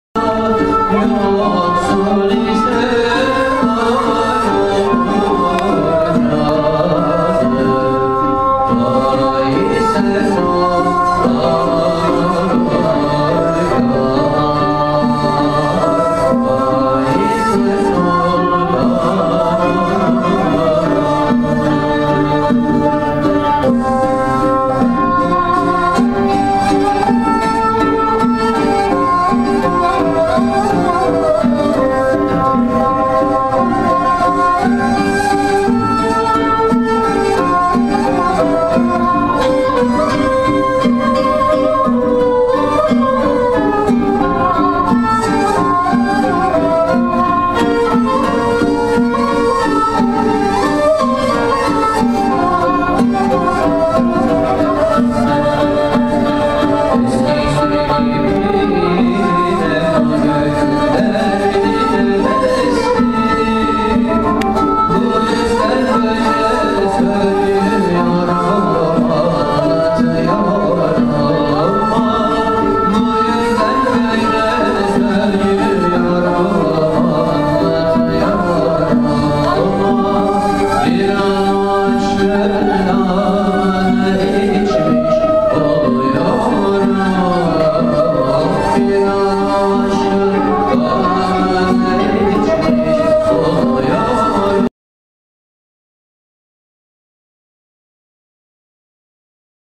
Whirling Dervish Ceremony Organisation by Les Arts Turcs
Dervish Ceremony in Paris - France / Theatre Chaillot by Les Arts Turcs.